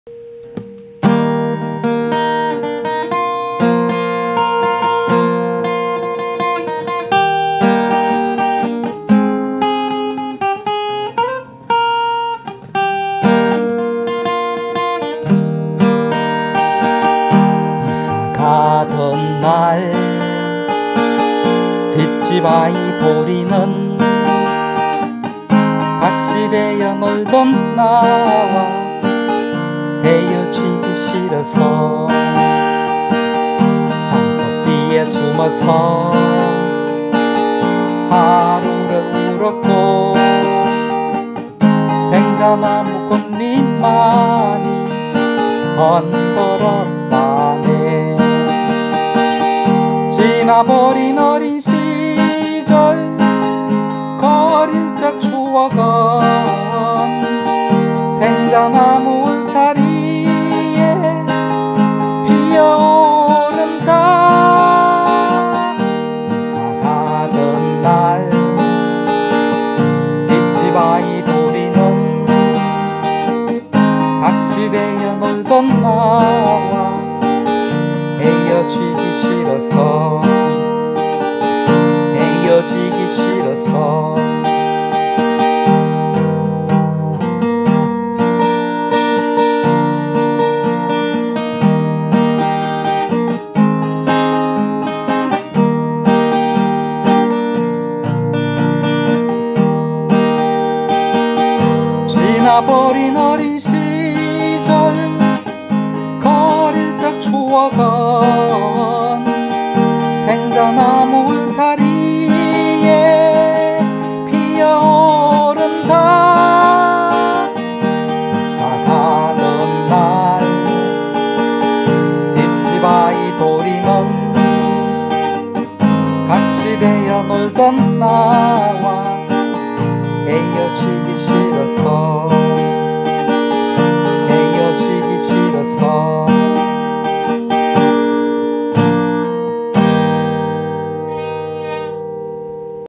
이사가던날
시골생활 4개월 정도 되었나 봅니다
모기에 시달리는 것도 조금씩 적응되고
좀 큰소리로 노래을 불러도 괜찮네요  ㅎㅎ